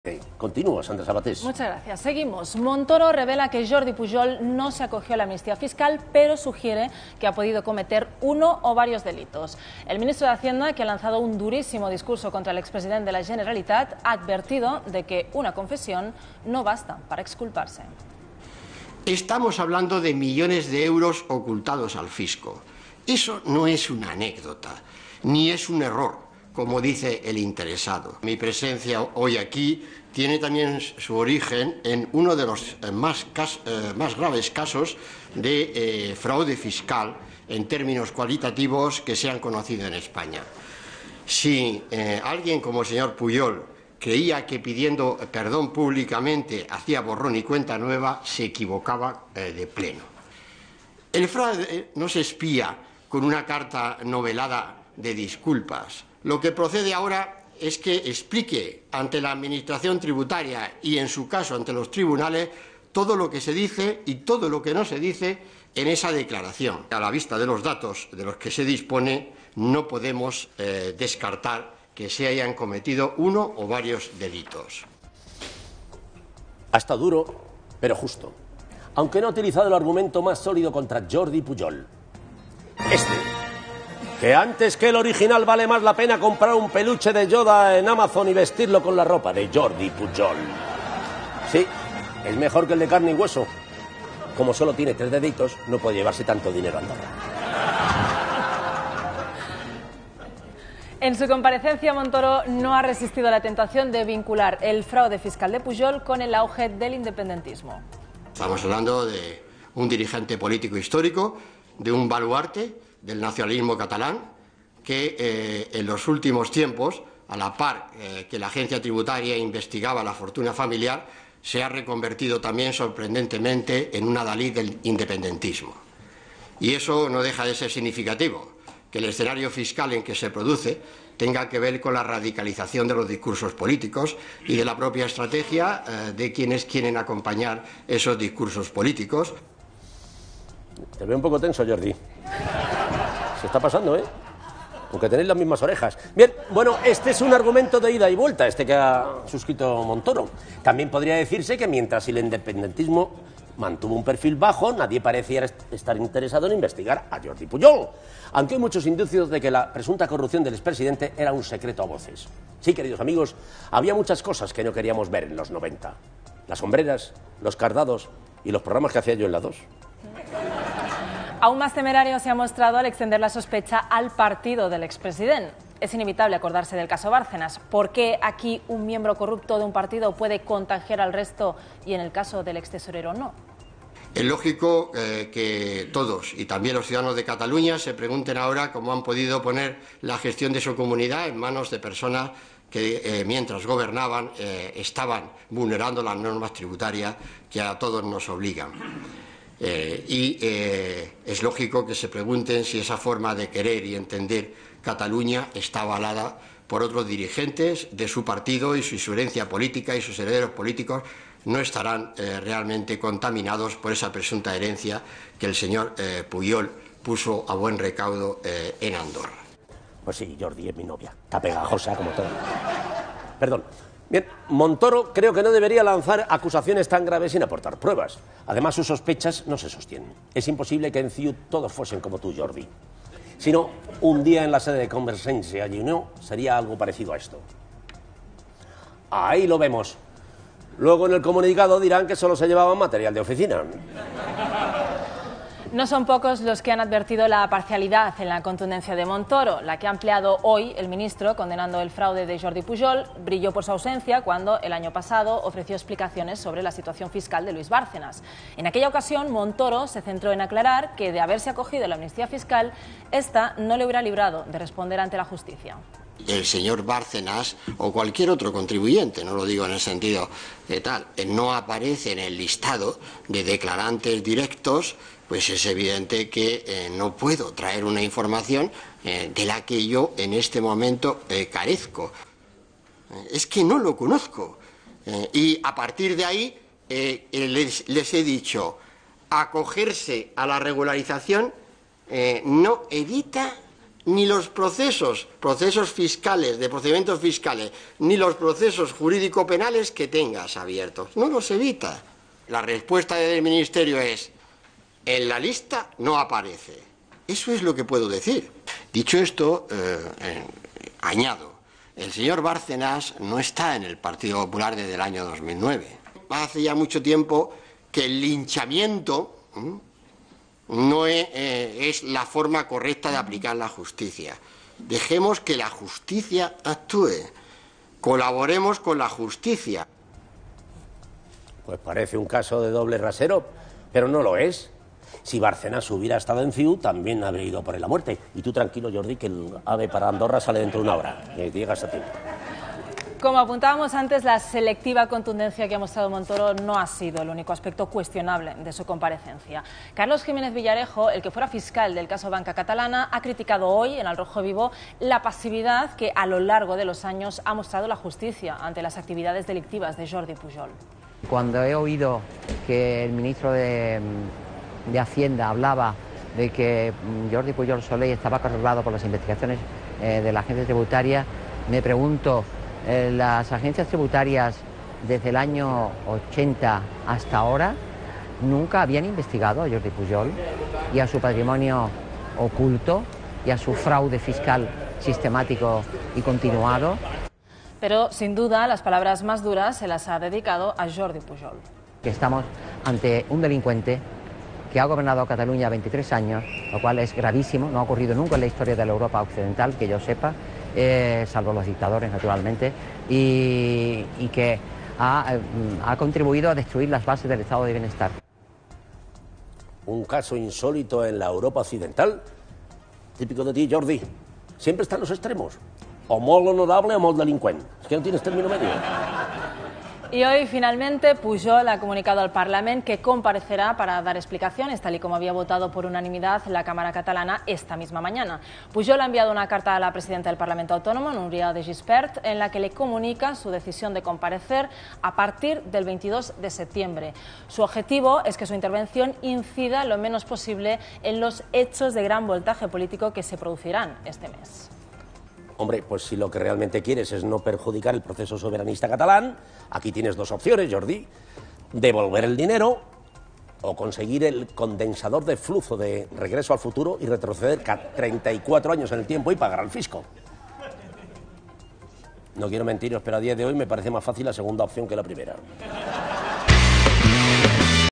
Wyoming: “Como sólo tiene tres deditos no puede llevarse tanto dinero a Andorra”
Cristóbal Montoro revela que Jordi Pujol no se acogió a la amnistía fiscal pero sugiere que ha podido cometer uno o varios delitos. El ministro de Hacienda que ha lanzado un durísimo discurso contra el expresident de la Generalitat ha advertido de que una confesión no basta para exculparse. Wyoming cree que las declaraciones de Montoro han sido justas pero duras. Aunque no ha utilizado el argumento más sólido contra Jordi Pujol: “Que antes que el original vale más la pena comprar un peluche de Yoda y vestirlo con la ropa de Jordi Pujol”, comenta el presentador.